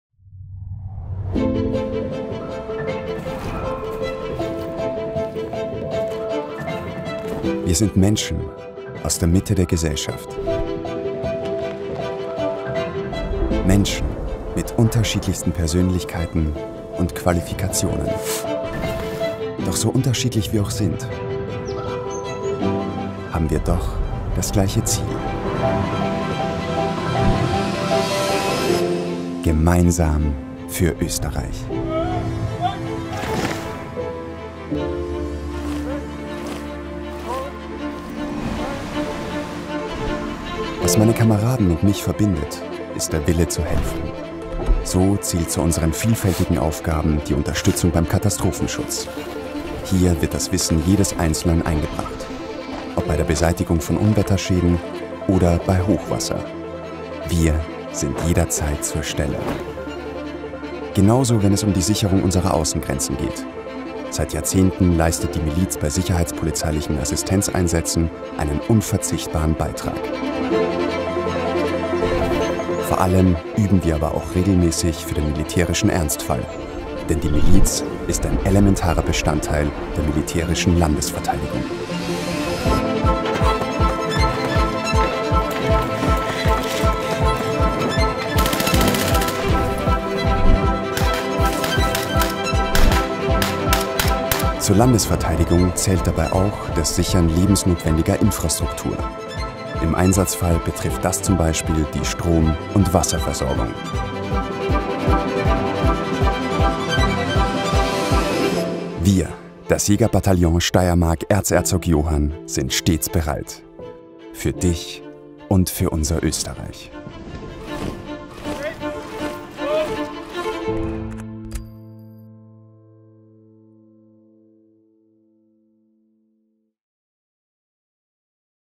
Das Jägerbataillon Steiermark
Die Miliz: Wir sind bereit, sei es bei Unwetterkatastrophen, bei der Grenzsicherung oder bei der militärischen Landesverteidigung unseren Beitrag zu leisten. Stets bereit für unser Österreich.